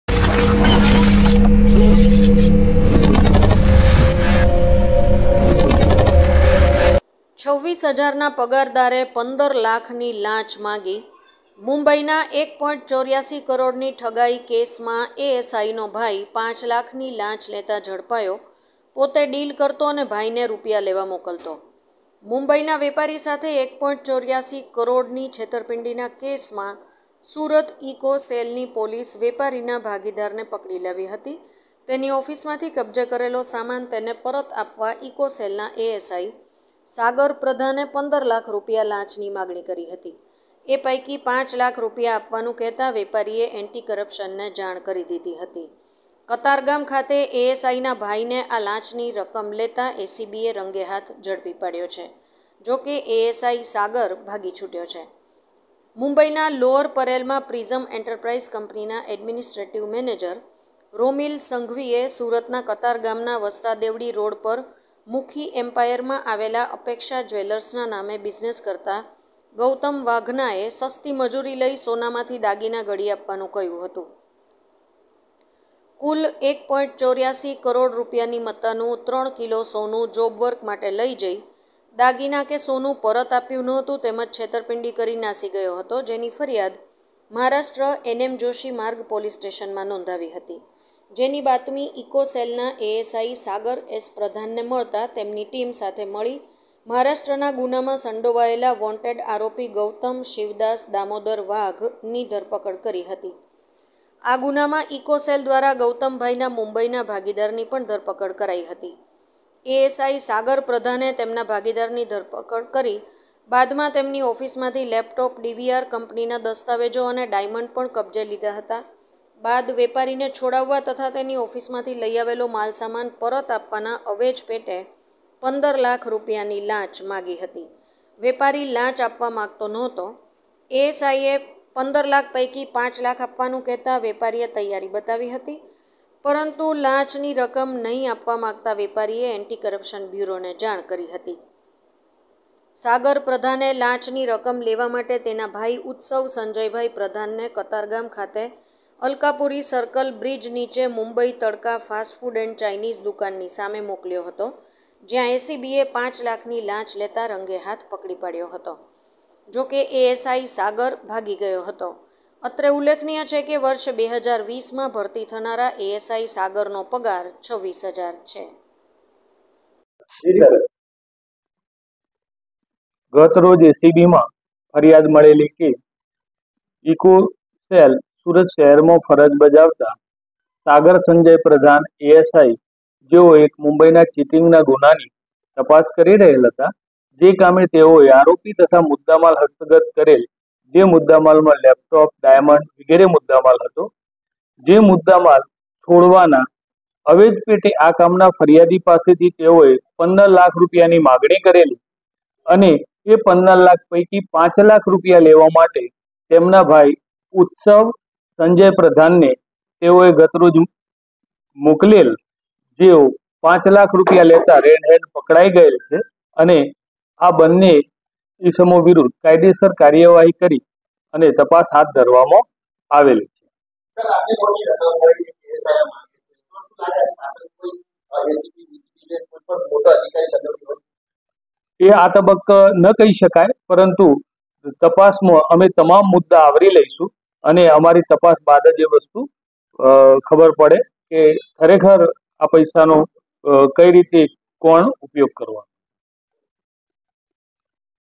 NEWS મુંબઈના 1.84 કરોડની ઠગાઈ કેસમાં ASIનો ભાઈ 5 લાખની લાંચ લેતાં ઝડપાયો,
26,000ના પગારદારે 15 લાખની લાંચ માગી..
મુંબઈના 1.84 કરોડની ઠગાઈ કેસમાં ASIનો ભાઈ 5 લાખની લાંચ લેતાં ઝડપાયો, પોતે ડીલ કરતો ને ભાઈને રૂપિયા લેવા મોકલતો..

મુંબઈના વેપારી સાથે 1.84 કરોડની છેતરપિંડીના કેસમાં સુરત ઈકો સેલની પોલીસ વેપારીના ભાગીદારને પકડી લાવી હતી. તેની ઓફિસમાંથી કબજે કરેલો સામાન તેને પરત આપવા ઈકો સેલના એએસઆઈ સાગર પ્રધાને 15 લાખ રૂપિયા લાંચની માગણી કરી હતી. એ પૈકી પાંચ લાખ રૂપિયા આપવાનું કહેતાં વેપારીએ એન્ટિકરપ્શનને જાણ કરી દીધી હતી. કતારગામ ખાતે એએસઆઈના ભાઈને આ લાંચની રકમ લેતાં એસીબીએ રંગે હાથ ઝડપી પાડ્યો છે. જોકે એએસઆઈ સાગર ભાગી છૂટ્યો છે. મુંબઈના લોઅર પરેલમાં પ્રીઝમ એન્ટરપ્રાઈઝ કંપનીના એડમિનિસ્ટ્રેટિવ મેનેજર રોમિલ સંઘવીએ સુરતના કતારગામના વસ્તાદેવડી રોડ પર મુખી એમ્પાયરમાં આવેલા અપેક્ષા જ્વેલર્સના નામે બિઝનેસ કરતા ગૌતમ વાઘનાએ સસ્તી મજૂરી લઇ સોનામાંથી દાગીના ઘડી આપવાનું કહ્યું હતું. કુલ 1.84 કરોડ રૂપિયાની મતાનું 3 કિલો સોનું જોબવર્ક માટે લઈ જઈ દાગીના કે સોનું પરત આપ્યું ન હતું તેમજ છેતરપિંડી કરી નાસી ગયો હતો, જેની ફરિયાદ મહારાષ્ટ્ર એન.એમ.જોષી માર્ગ પોલીસ સ્ટેશનમાં નોંધાવી હતી. જેની બાતમી ઈકો સેલના એએસઆઈ સાગર એસ.પ્રધાનને મળતાં તેમની ટીમ સાથે મળી મહારાષ્ટ્રના ગુનામાં સંડોવાયેલા વોન્ટેડ આરોપી ગૌતમ શિવદાસ દામોદર વાઘ (ઉં.વ.32) (રહે., ભવ્યરાજ રેસિડેન્સી, પાલ)ની ધરપકડ કરી હતી. આ ગુનામાં ઈકો સેલ દ્વારા ગૌતમભાઈના મુંબઈના ભાગીદારની પણ ધરપકડ કરાઈ હતી. એએસઆઈ સાગર પ્રધાને તેમના ભાગીદારની ધરપકડ કરી બાદમાં તેમની ઓફિસમાંથી લેપટોપ, ડીવીઆર, કંપનીના દસ્તાવેજો અને ડાયમંડ પણ કબજે લીધાં હતાં.બાદ વેપારીને છોડાવવા તથા તેની ઓફિસમાંથી લઈ આવેલો માલ-સામાન પરત આપવાના અવેજ પેટે 15 લાખ રૂપિયાની લાંચ માગી હતી. વેપારી લાંચ આપવા માગતો નહોતો. એએસઆઈએ 15 લાખ પૈકી 5 લાખ આપવાનું કહેતાં વેપારીએ તૈયારી બતાવી હતી, પરંતુ લાંચની રકમ નહીં આપવા માગતા વેપારીએ એન્ટિકરપ્શન બ્યૂરોને જાણ કરી હતી.સાગર પ્રધાને લાંચની રકમ લેવા માટે તેના ભાઈ ઉત્સવ સંજયભાઈ પ્રધાનને કતારગામ ખાતે અલકાપુરી સર્કલ બ્રિજ નીચે મુંબઈ તડકા ફાસ્ટ ફૂડ એન્ડ ચાઈનીઝ દુકાનની સામે મોકલ્યો હતો, જ્યાં એસીબીએ 5 લાખની લાંચ લેતાં રંગેહાથ પકડી પાડ્યો હતો. જોકે એએસઆઈ સાગર ભાગી ગયો હતો. અત્રે ઉલ્લેખનીય છે કે વર્ષ 2020માં ભરતી થનારા ASI સાગરનો પગાર 26 હજાર છે.